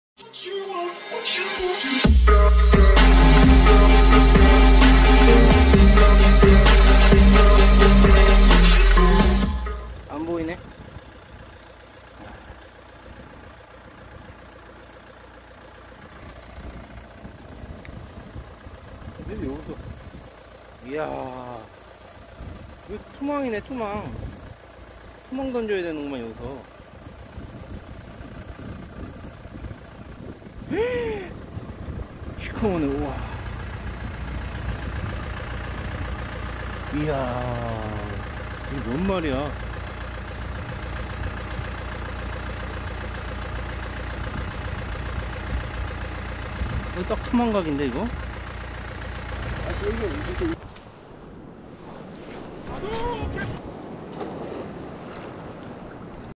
2020년 11월 29일 도루묵 오호항 상황
도루묵 백만대군 입성이요~

오호항  3시출발 6시도착 9시까지조업 퇴근3시간...

시간당100kg  세시간 300kg

아들이랑 통발두개로  잡고왔습니다

모두 나눔하고  이제쉽니다.